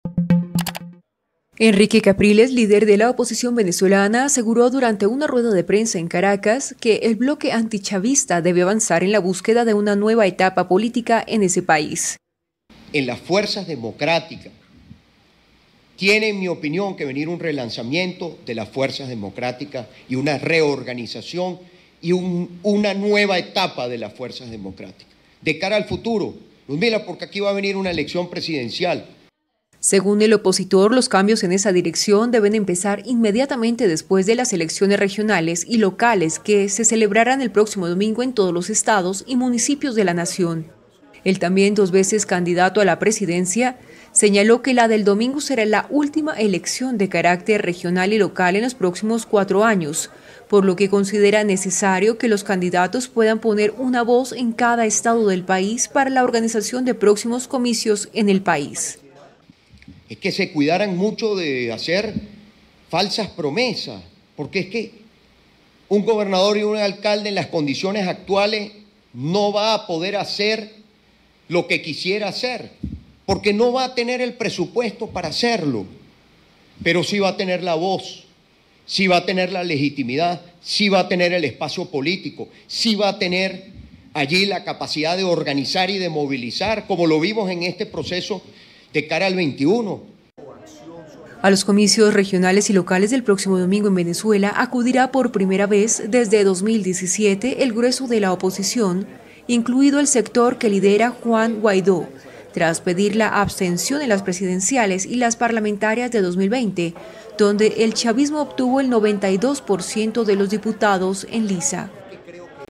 Capriles llama a antichavistas a crear una nueva era en Venezuela
El líder de la oposición venezolana Henrique Capriles aseguró este viernes que el bloque antichavista debe ir a un proceso de relanzamiento y reorganización de cara al futuro y en la búsqueda de una nueva etapa política en el país caribeño.

Según el opositor, los cambios en esa dirección deben empezar inmediatamente después de las elecciones regionales y locales que se celebrarán el próximo domingo en todos los estados y municipios de la nación.